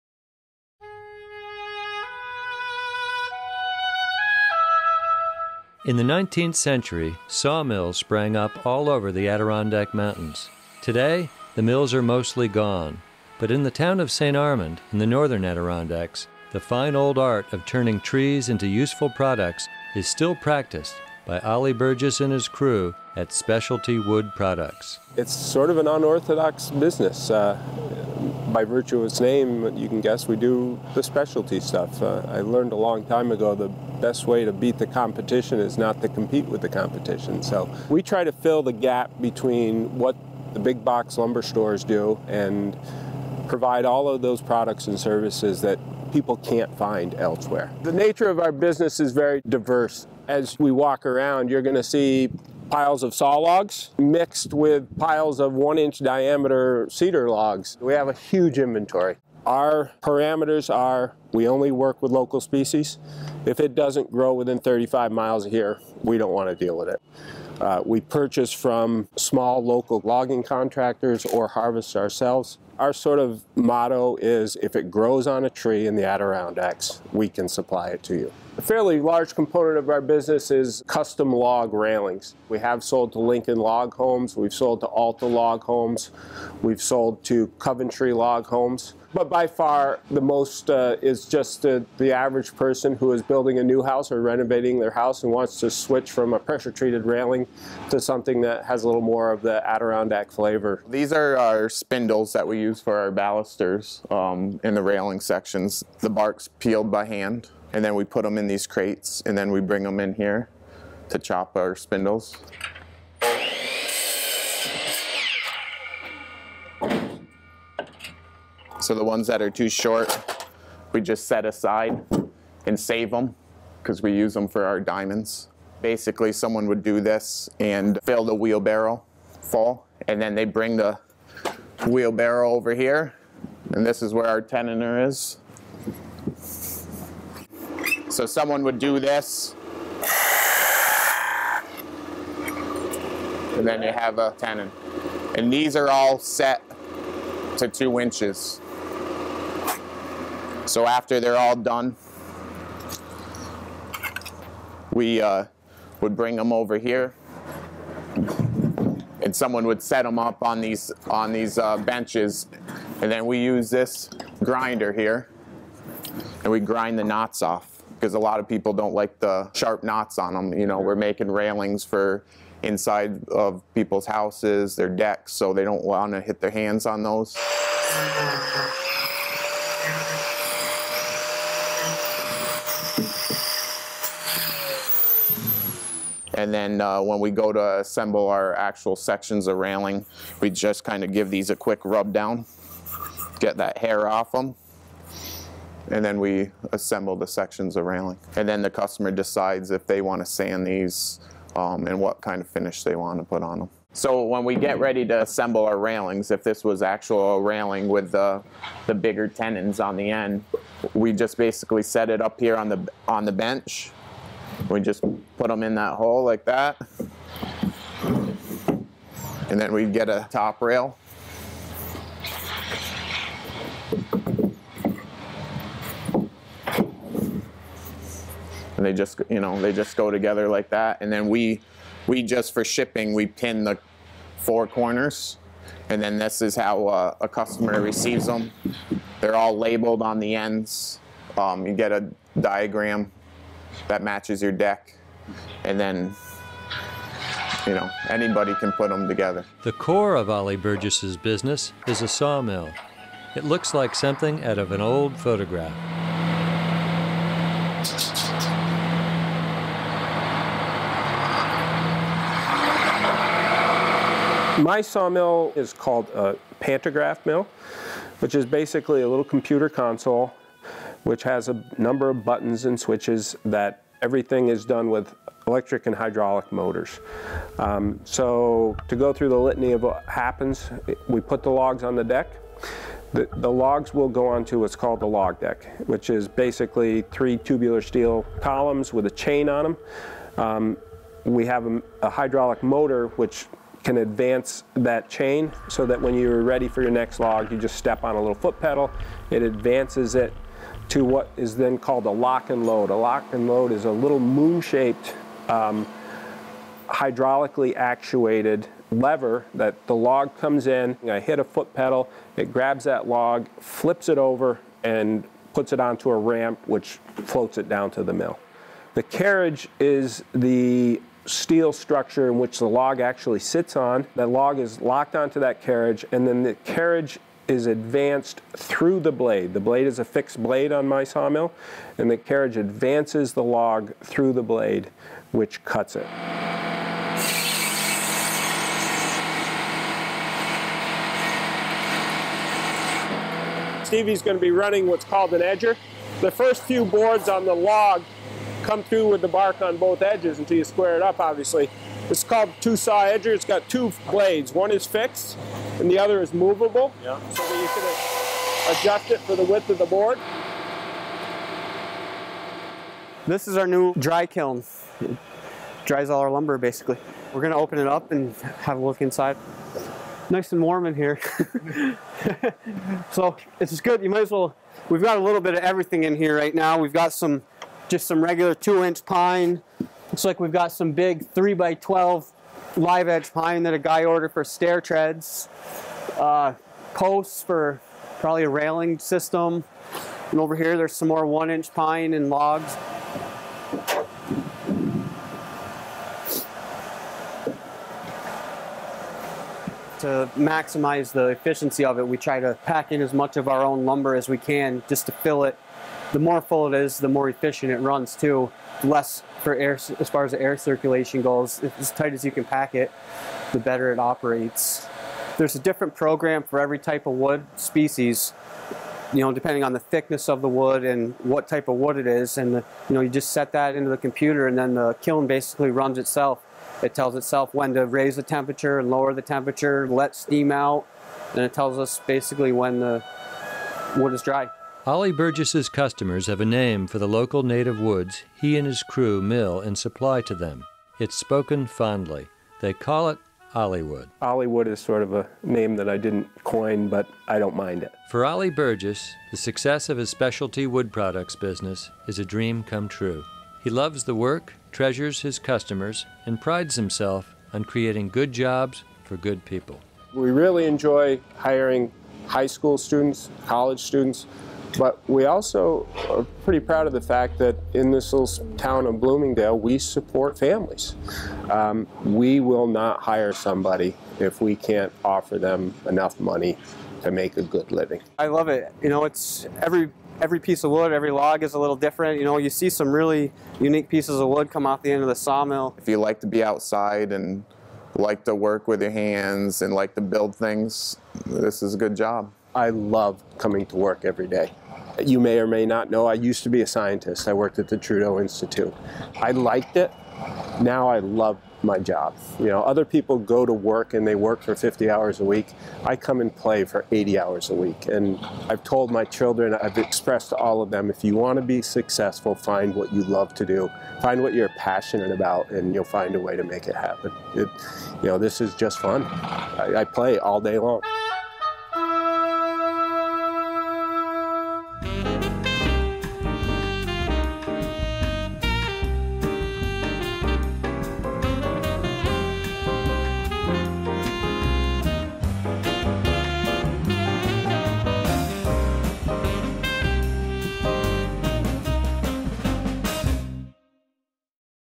Going "Ollywood": How A Modern Adirondack Sawmill Produces Old-Time Forest Products
A scientist turned sawyer named Ollie Burgess turns trees into rustic building materials in the northern Adirondack township of St. Armand. It's a bustling business, on one hand appearing straight out of the nineteenth century and on the other incorporating modern technology at every turn. Whether it's rough-sawn native lumber or a mortise-and-tenon-jointed cedar railing with the bark on, if it's on a delivery truck bound for a building site in the United States, it's likely point of origin was Burgess's Specialty Wood Products.